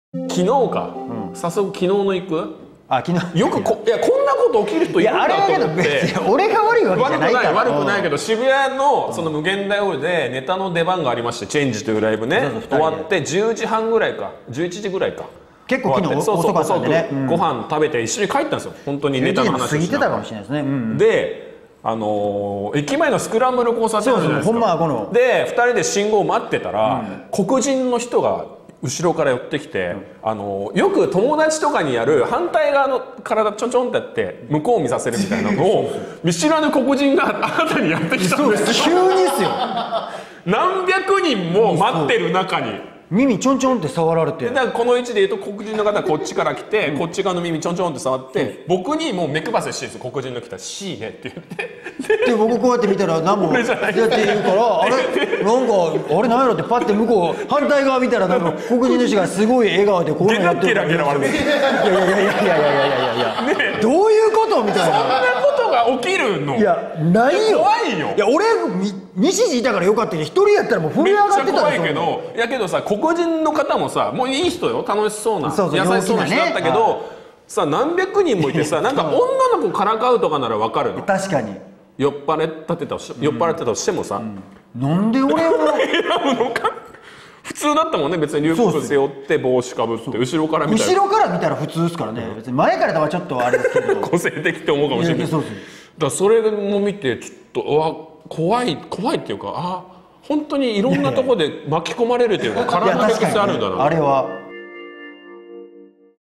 渋谷の交差点で外国人から悪戯されて… よしログ
2018年10月03日（水）シンガリのよしログ。東京・渋谷のスクランブル交差点で信号待ちしていたシンガリの2人。すると滝野の背後に立っていた見知らぬ外国人が、滝野の耳を触っていたずらを仕掛けてきたという。いたずらをされた滝野は……。